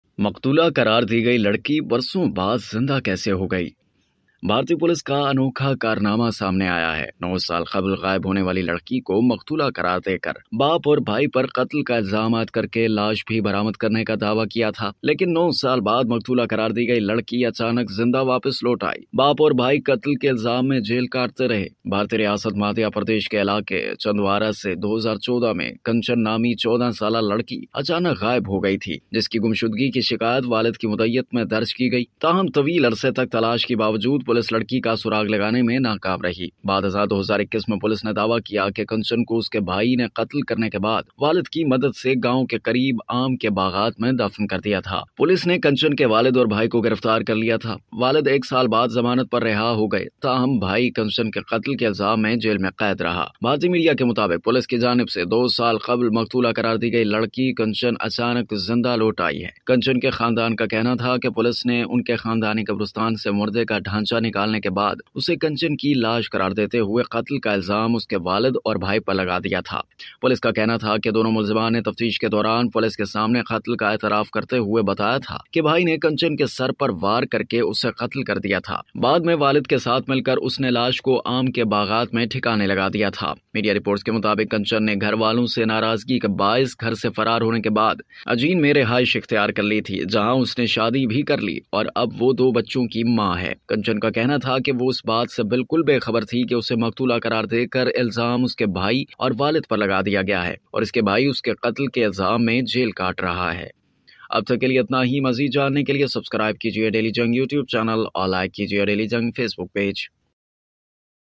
Madhya Pradesh girl resurfaces years after father and brother jailed for her murder
‘Murdered’ girl returns home to find dad and  brother jailed for killing her in Madhya Pradesh's Chhindwara